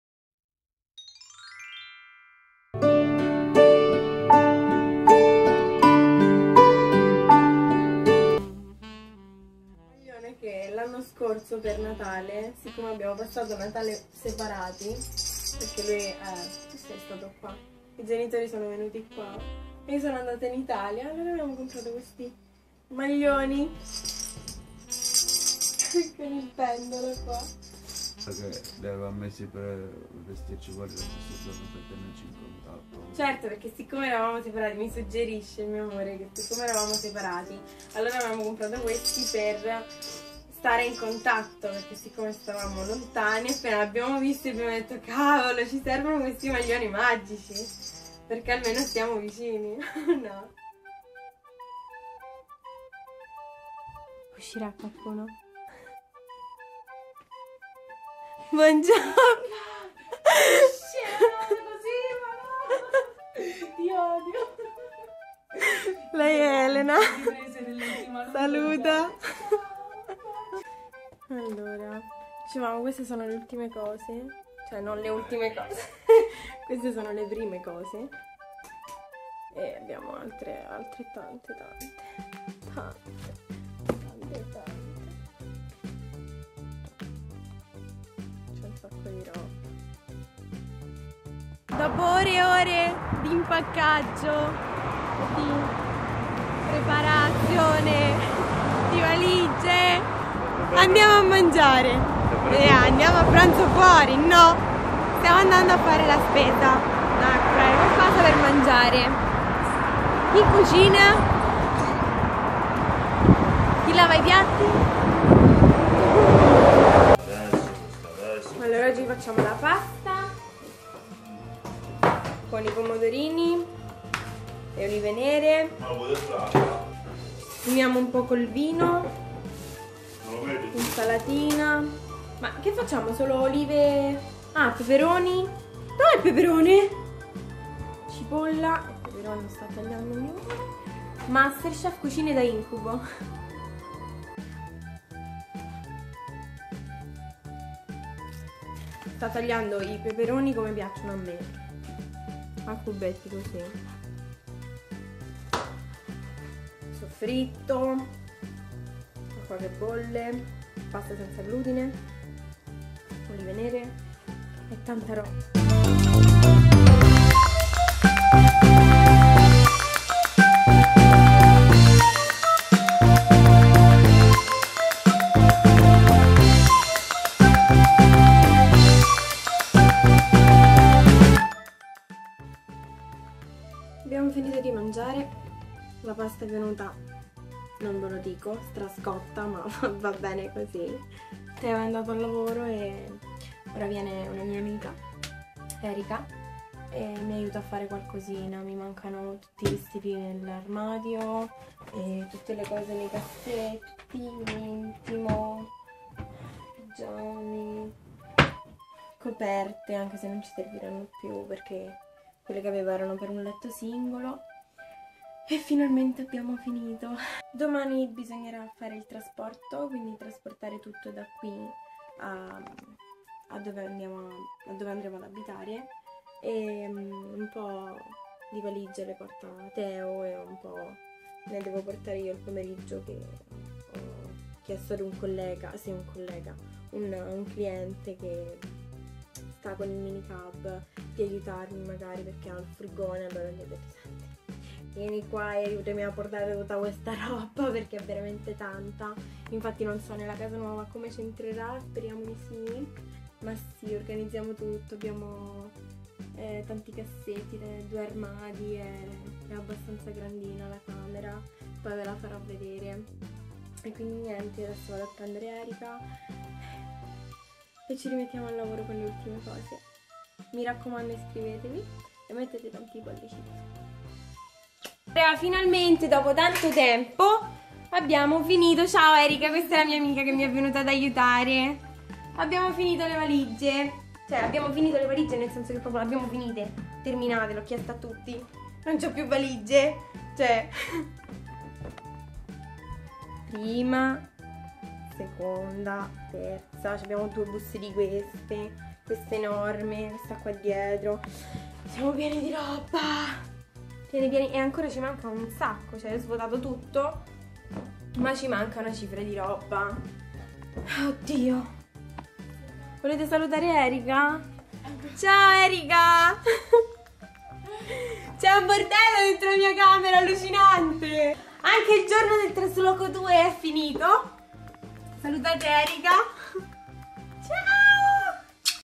LONDRA - TRASLOCO 2.0 || Cucine Da Incubo - Come Non Fare un trasloco Day 2!
Ci siamo improvvisati chef, non è andato proprio tutto storto. !!! ahahahah
METTETE TANTI MI PIACE
E RICORDATEVI SE VA MALE IL PIANO A ESISTE SEMPRE IL PIANO B